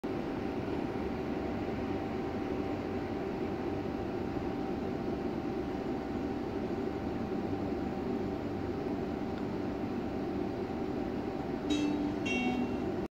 Sydney Trains cover your nose and mouth when coughing or sneezing
Use a tissue or a flexed elbow to avoid spread of Covid 19 Corona virus